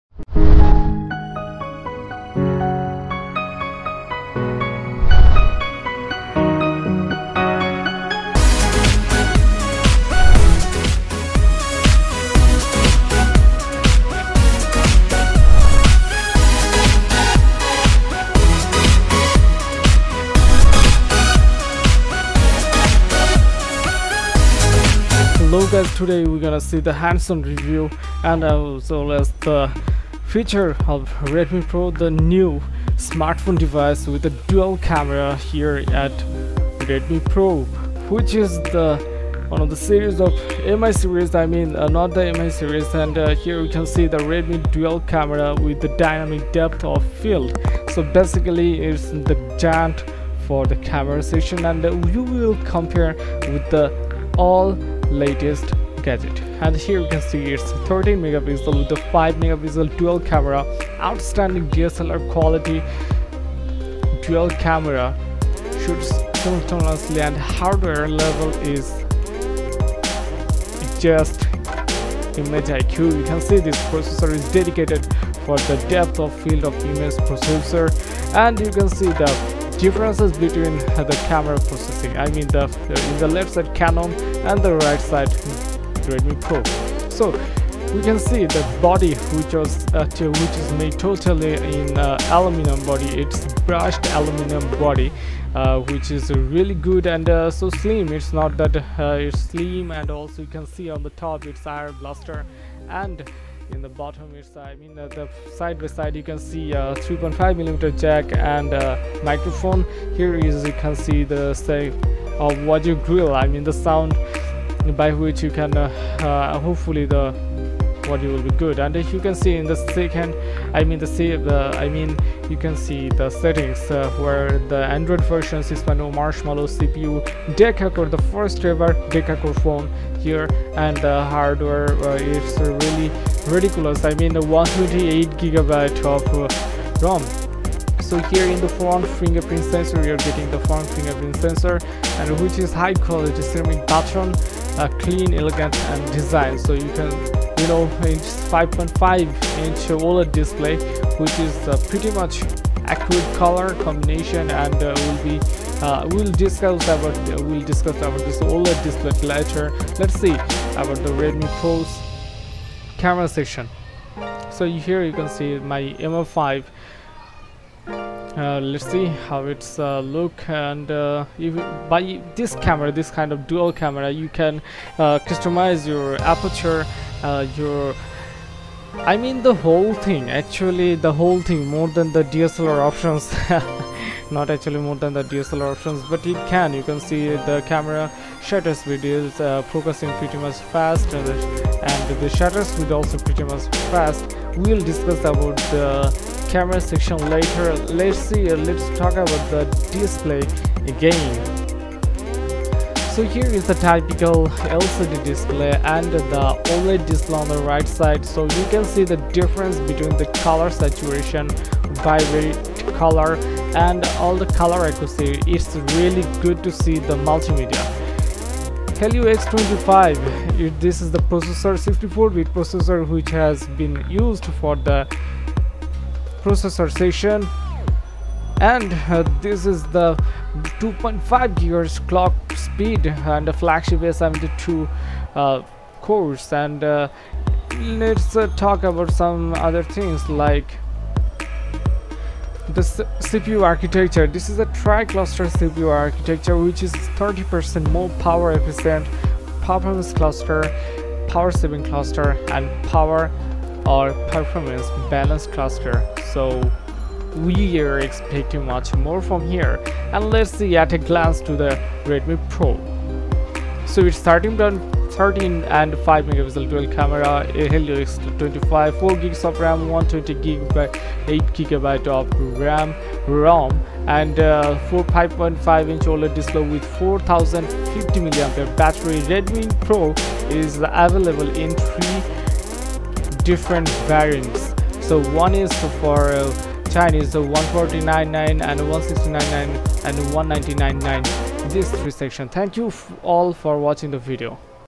Xiaomi Redmi Pro | Exclusive hands on review
The Redmi Pro  houses a deca core MediaTek Helio X20 MT6757 system on a chip. Its CPU runs at 2.1 GHz, which is much higher than average for a smartphone processor!!